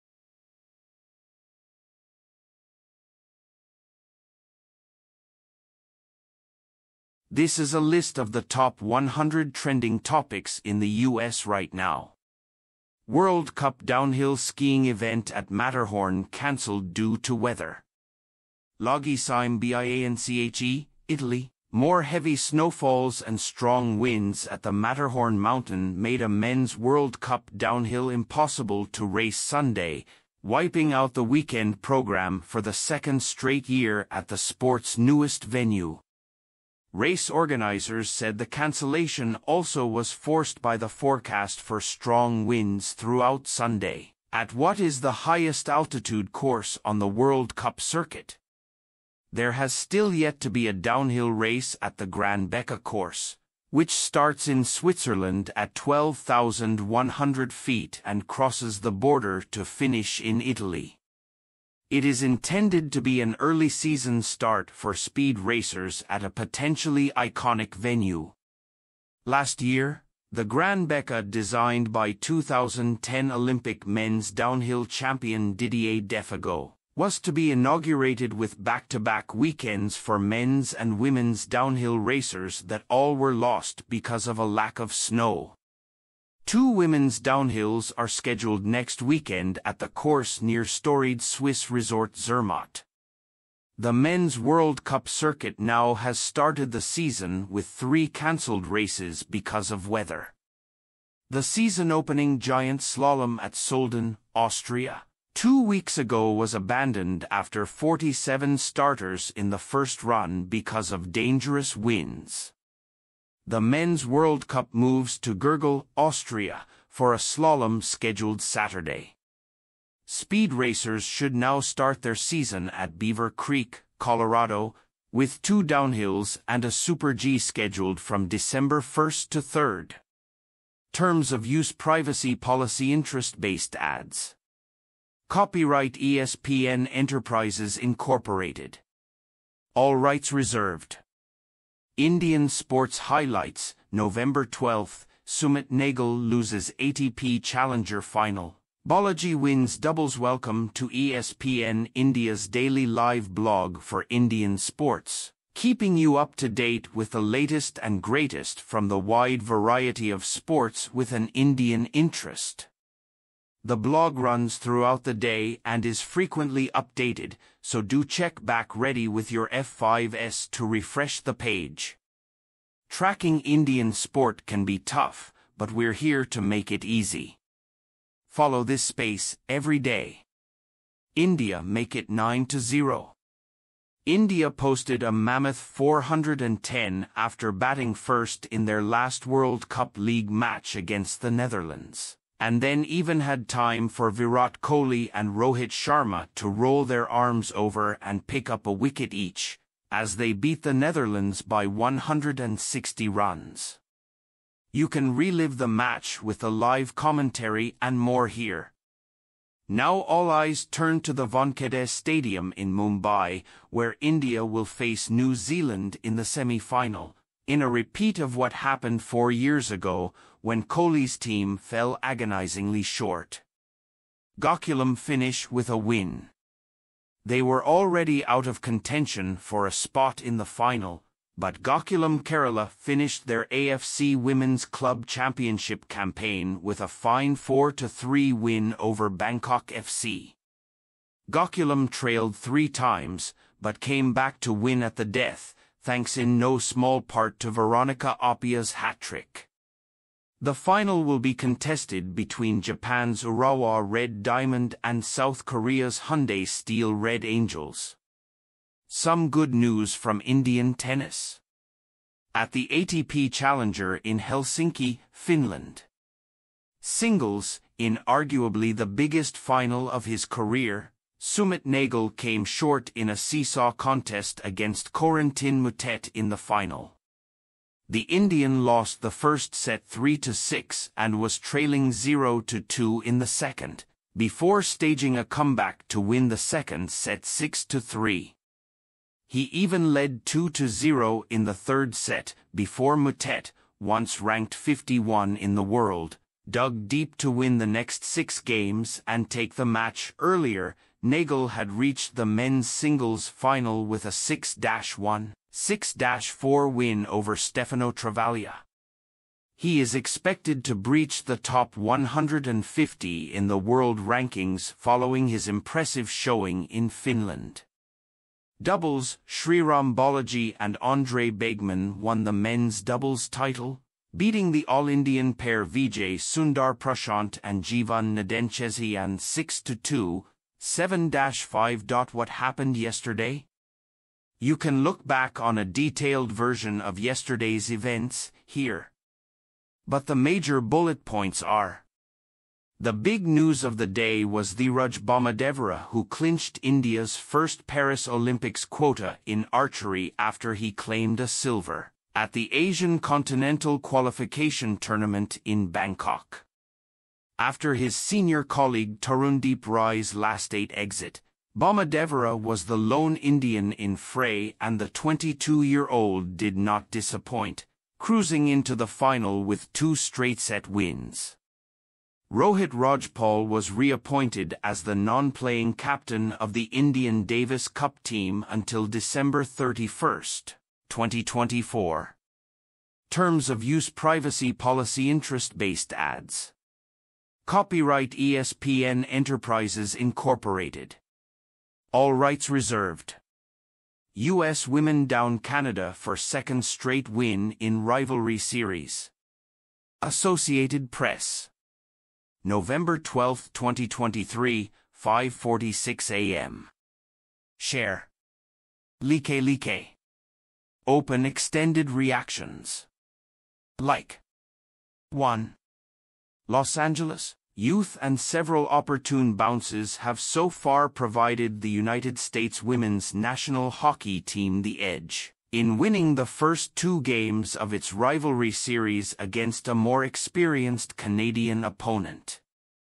World Cup downhill skiing event at Matterhorn canceled due to weather
LAGHI CIME BIANCHE, Italy -- More heavy snowfalls and strong winds at the Matterhorn mountain made a men's World Cup downhill impossible to race Sunday, wiping out the weekend program for the second straight year at the sport's newest venue.